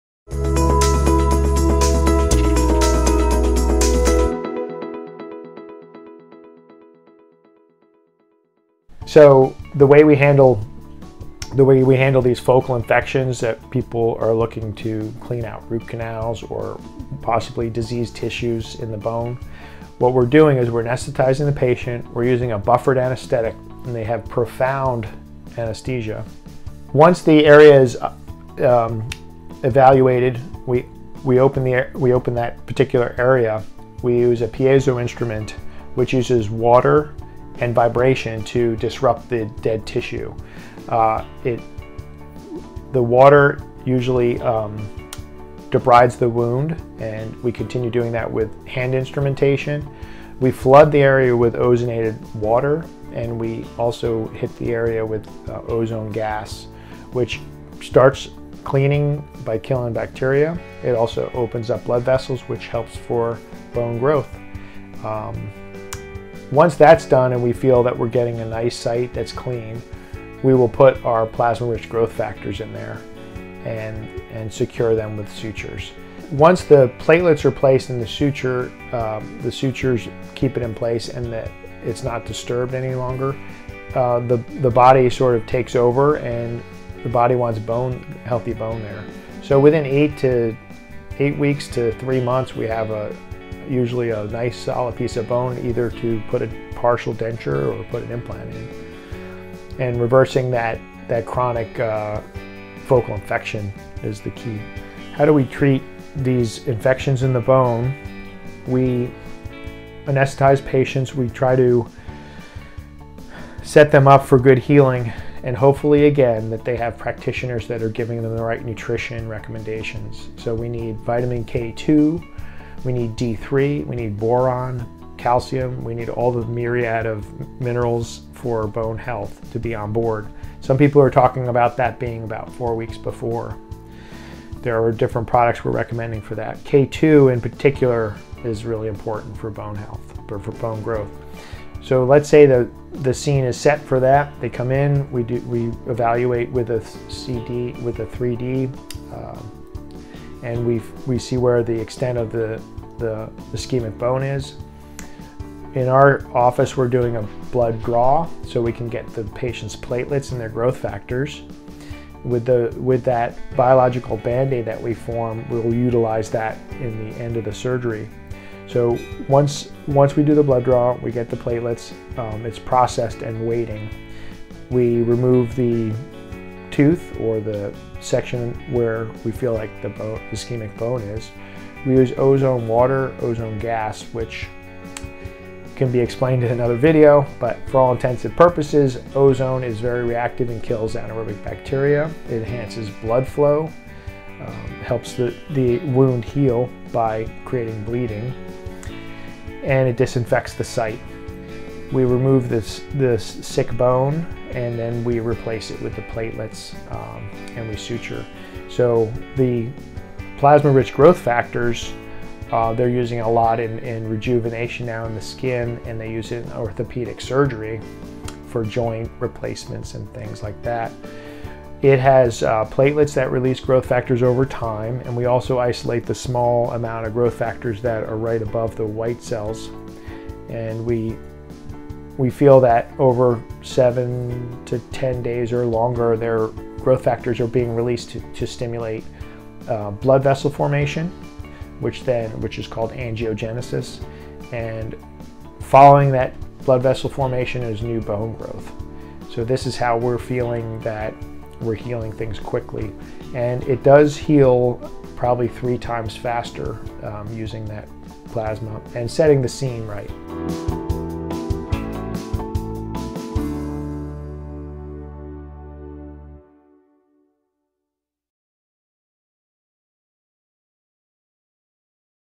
Dental Wellness Center Dealing with Focal Infections
Infections in the jaw bone are observed and handled everyday at the Dentalwellnesscenter
PRGF, PRF, Platelet rich growth factors, ozone, Piezotome, angiogenesis, focal infections.
Vitamin K2, Vitamin D3. Osteonecrosis of jaw bone